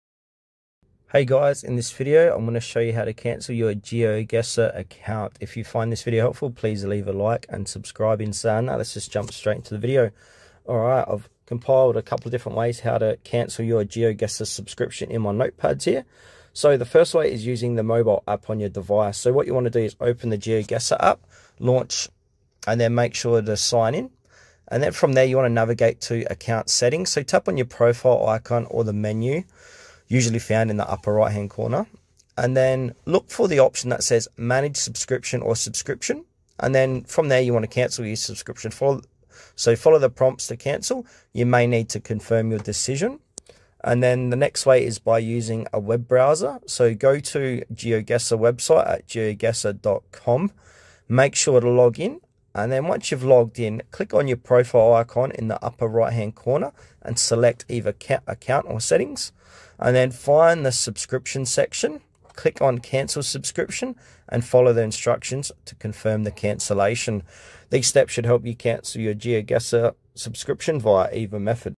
How To Cancel Geoguessr Subscription
In this video, I’ll guide you through the steps to cancel your GeoGuessr subscription easily and quickly. Whether you’re taking a break or ending your subscription, this step-by-step tutorial will help you navigate the cancellation process smoothly. Follow along to manage your GeoGuessr subscription with ease!